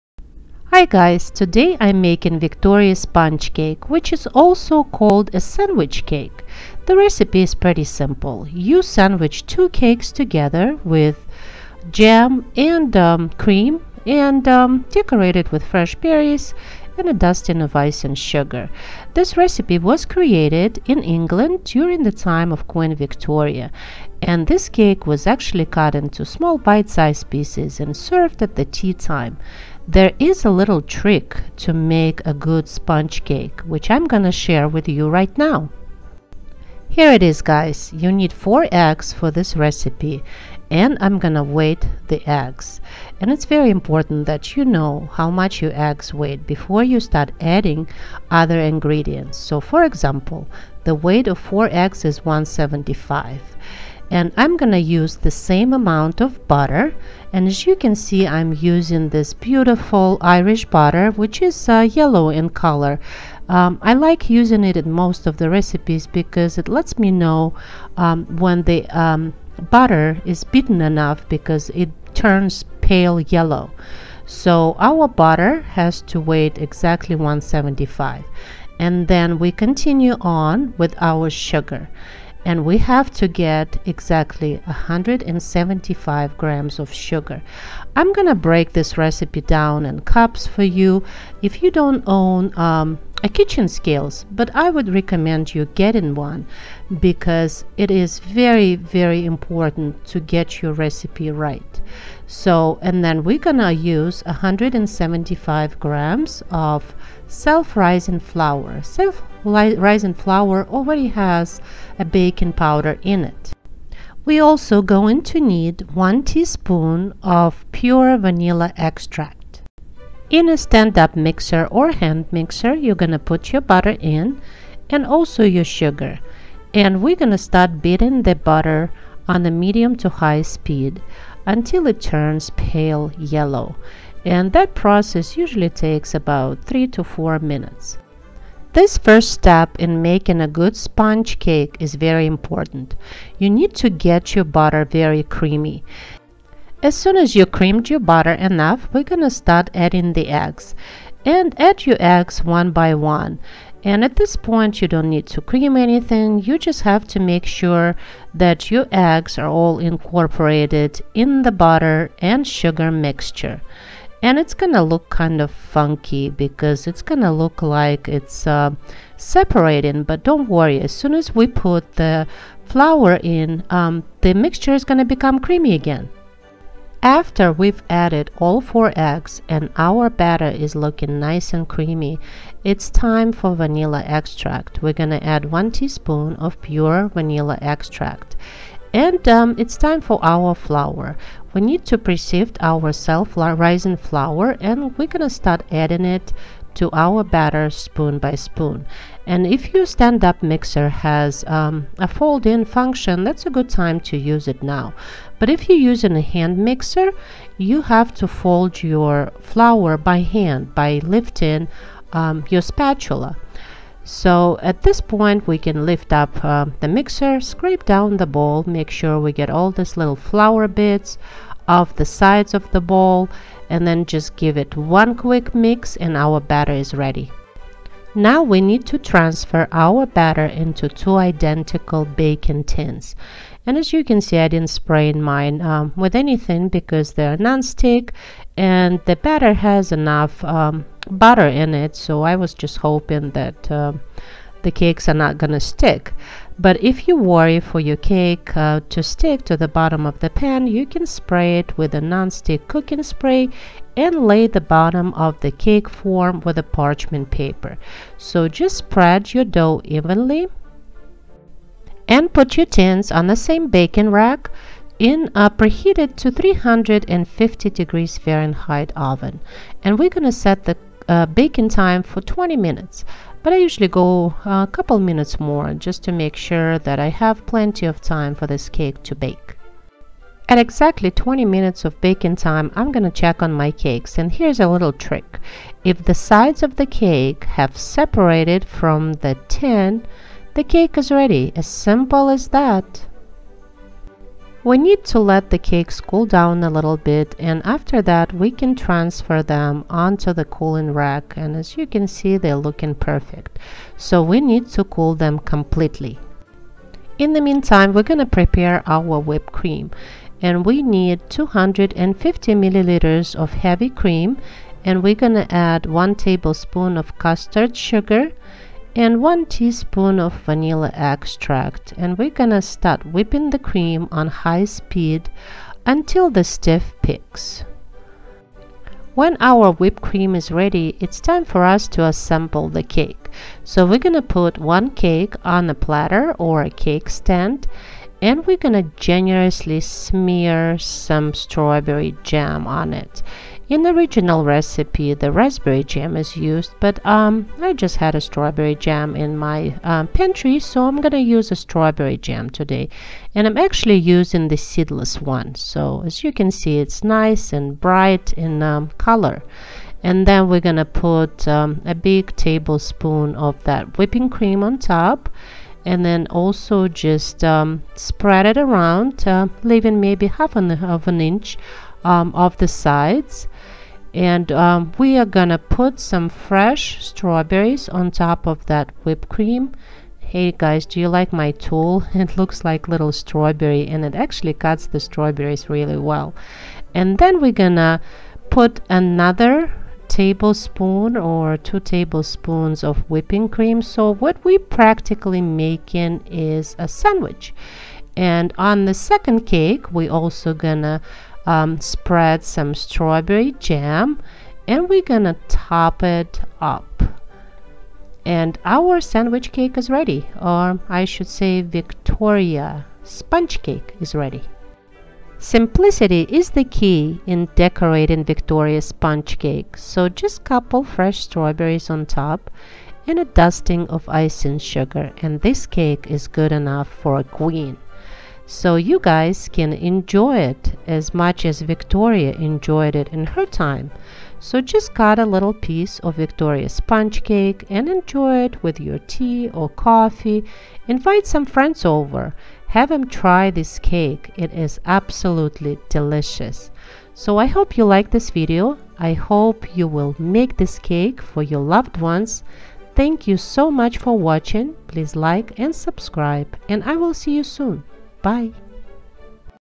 Victoria Sponge Cake-Good Enough for a Queen!
The Victoria Sponge Cake is two sponge cakes sandwiched together with jam and cream.
This recipe was created in England, by one of the Queen Victoria's lady-in-waiting. This cake was cut into bite size pieces and served at the "Tea Time".
The rule of making moist sponge cake is simple: WEIGHT EGGS FIRST! Yes, when you know the weight of 4 eggs needed for this recipe, you just add the same exact amount of butter, sugar and self-rising flour!
As simple as that!

For sponges you will need:

4 eggs (175g) (the weight could very depends on the size)
175g (3/4 cup) (6oz) soften butter
175g (1 cup) (6oz) superfine sugar
175g (1 cup) (6oz) self-rising flour
1 teaspoon of pure vanilla extract

Method:

Cream butter and sugar for 4-5 min until smooth and pale yellow.
Gradually add 4 eggs. Mix well.
Fold in self-rising flour just to combine.
Add vanilla.

Divide batter into two 9 inch baking tins. (You need to grease them generously or line with parchment paper, if using aluminum ones) Spread batter evenly.

Bake your cakes in preheated to 350°F oven for 20 minutes or until cakes have separated from sides of baking tins.
Let cool.
Whip up 250ml of heavy cream with 1 tablespoon of super-fine sugar and 1 teaspoon of vanilla.

Assemble by sandwiching two cakes with spread of strawberry jam, layer of whipped cream and fresh strawberries.

Decorate your Victoria Sponge Cake with fresh strawberries and a dust of icing sugar!

Optional: you can use raspberry jam and fresh raspberries!

Nastassja :)